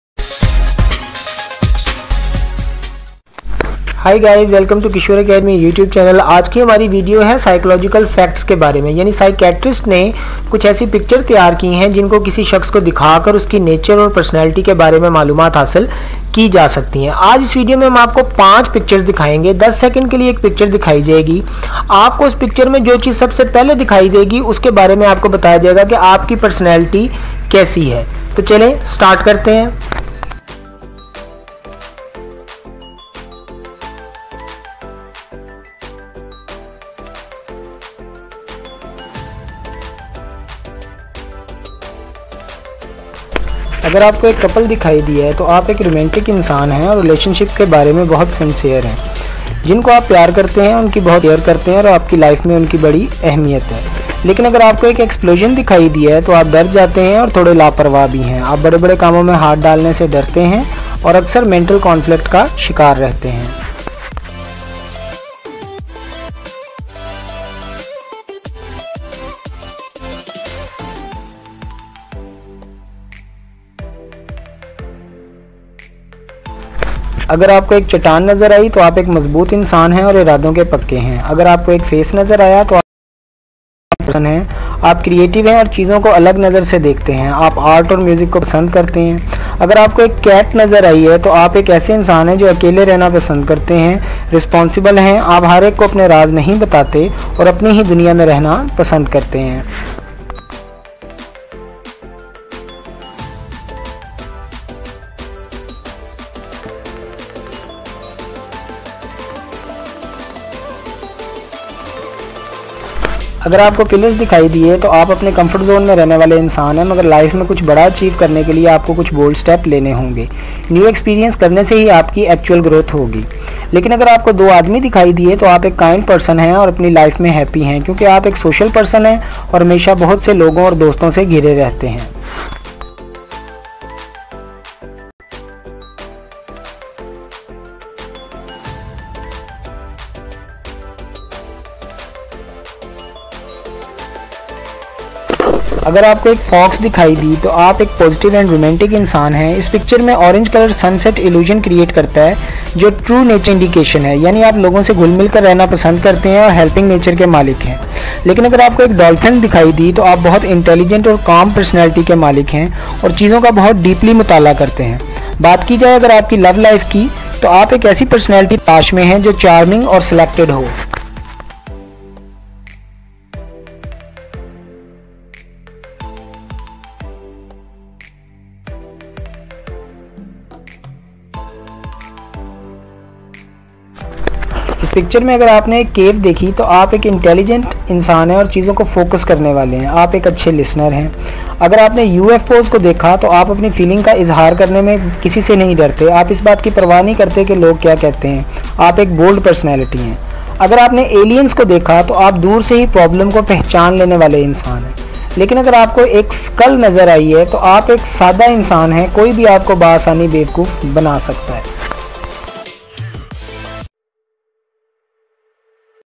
5 Mind Blowing psychology Test | Personality Test
Watch psychological facts about your personality .
Mind blowing techniques use by psychiatrist to check a personality of a person .